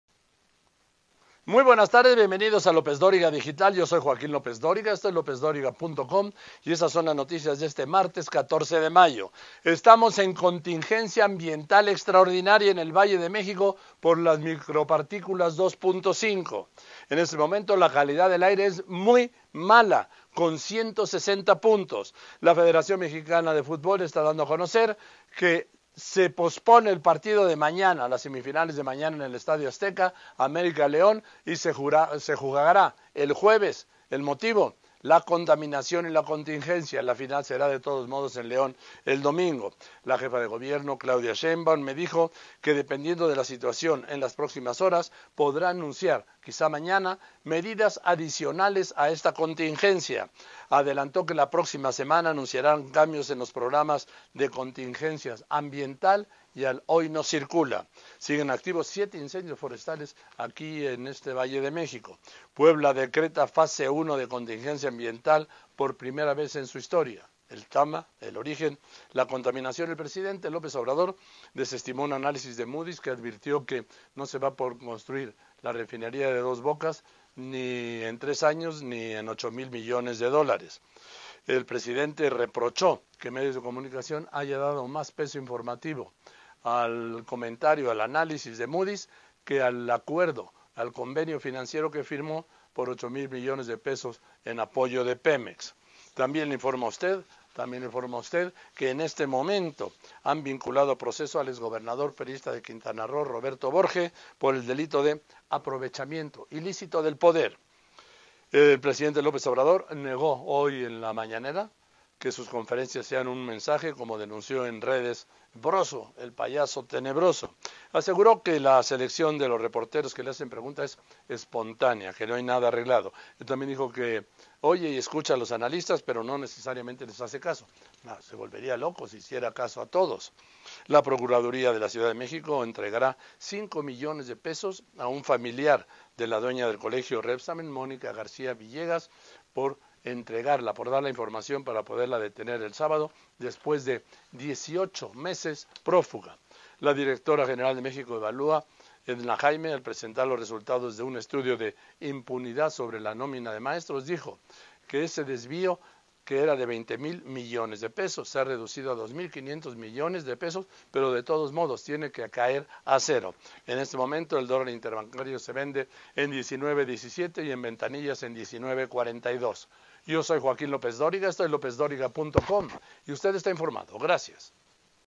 ¡Las noticias! Estamos en contingencia ambiental extraordinaria, la calidad del aire es muy mala
¡Las noticias! Estamos en contingencia ambiental extraordinaria, la calidad del aire es muy mala; cambian de fecha partido entre América y León en el Azteca por contaminación; siguen activos siete incendios forestales en el Valle de México, y más...